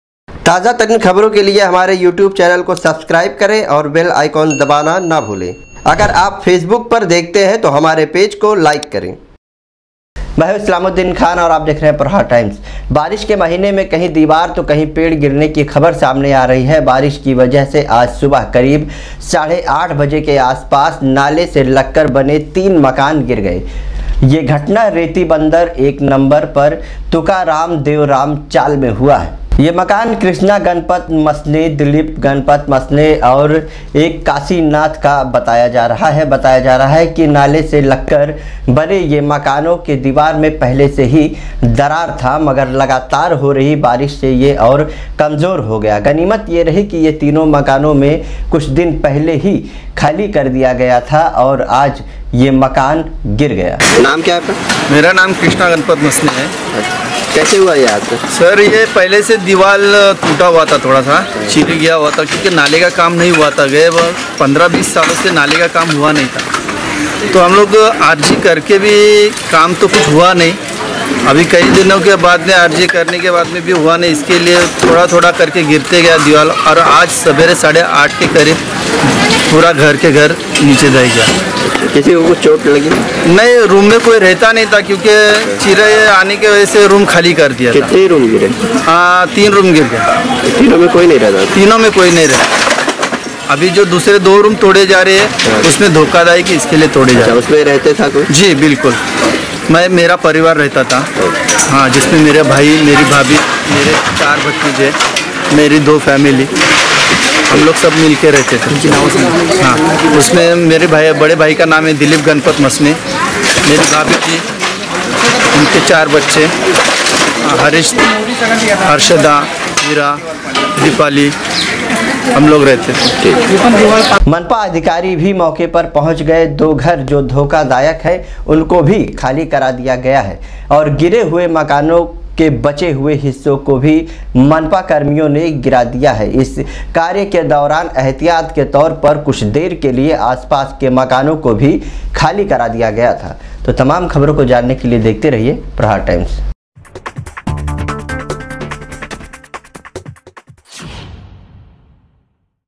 रेती बंदर नाले पर बने तीन मकान आज सुबह गिर गए ,Prahar Times
रेती बंदर नाले पर बने तीन मकान आज सुबह गिर गए
रेती बंदर एक नंबर में सुबह 8:00 बजे हुआ हादसा

हादसे में किसी के जख्मी होने की खबर नहीं

मौके पर पहुंची मनपा कर्मियों ने धोखा दायक मकान खाली कराया

prahar times